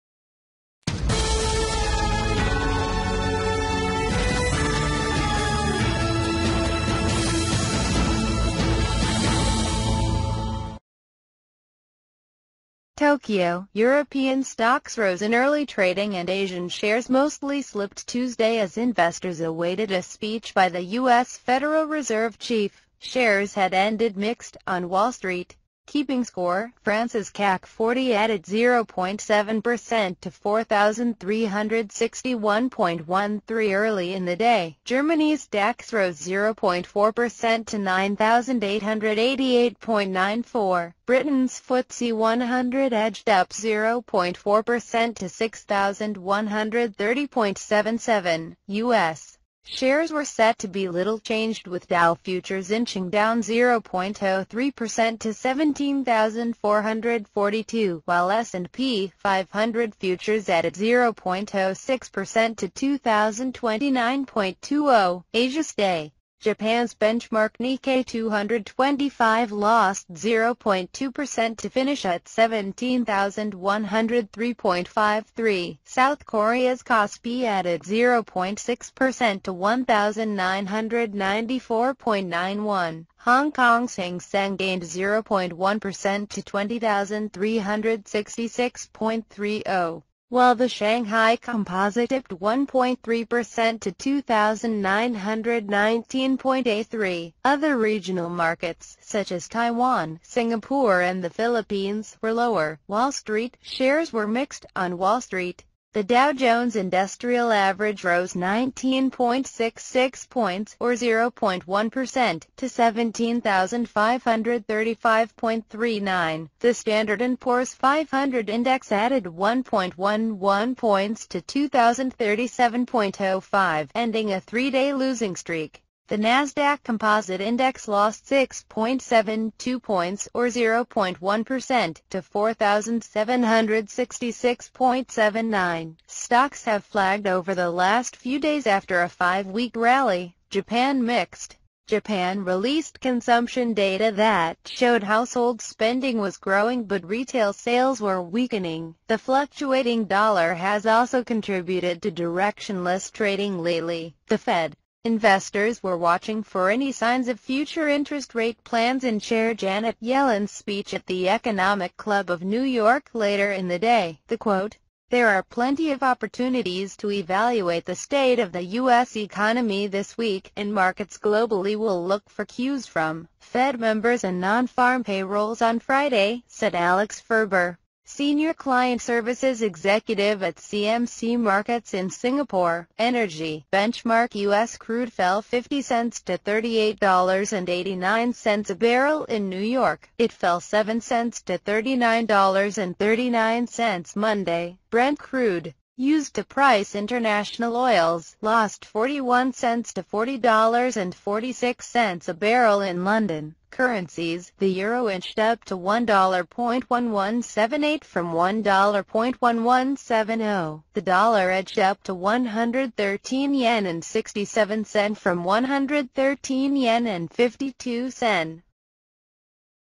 Europe higher, Asia mostly lower before Fed chair speech
OKYO - European stocks rose in early trading and Asian shares mostly slipped Tuesday as investors awaited a speech by the U.S. Federal Reserve chief. Shares had ended mixed on Wall Street.

KEEPING SCORE: France's CAC 40 added 0.7 per cent to 4,361.13 early in the day. Germany's DAX rose 0.4 per cent to 9,888.94. Britain's FTSE 100 edged up 0.4 per cent to 6,130.77. U.S. shares were set to be little changed with Dow futures inching down 0.03 per cent to 17,442, while S&P 500 futures added 0.06 per cent to 2,029.20.

ASIA'S DAY: Japan's benchmark Nikkei 225 lost 0.2 per cent to finish at 17,103.53. South Korea's Kospi added 0.6 per cent to 1,994.91. Hong Kong's Hang Seng gained 0.1 per cent to 20,366.30, while the Shanghai Composite dipped 1.3 per cent to 2,919.83. Other regional markets, such as Taiwan, Singapore and the Philippines, were lower.

WALL STREET: Shares were mixed on Wall Street. The Dow Jones industrial average rose 19.66 points, or 0.1 per cent, to 17,535.39. The Standard & Poor's 500 index added 1.11 points to 2,037.05, ending a three-day losing streak. The Nasdaq composite index lost 6.72 points, or 0.1 per cent, to 4,766.79. Stocks have flagged over the last few days after a five-week rally.

JAPAN MIXED: Japan released consumption data that showed household spending was growing but retail sales were weakening. The fluctuating dollar has also contributed to directionless trading lately.

THE FED: Investors were watching for any signs of future interest rate plans in Chair Janet Yellen's speech at the Economic Club of New York later in the day.

THE QUOTE: "There are plenty of opportunities to evaluate the state of the U.S. economy this week, and markets globally will look for cues from ... Fed members and non-farm payrolls on Friday," said Alex Furber, senior client services executive at CMC Markets in Singapore.

ENERGY: Benchmark U.S. crude fell 50 cents to $38.89 a barrel in New York. It fell 7 cents to $39.39 Monday. Brent crude, used to price international oils, lost 41 cents to $40.46 a barrel in London.

CURRENCIES: The euro inched up to $1.1178 from $1.1170. The dollar edged up to 113.67 yen from 113.52 yen.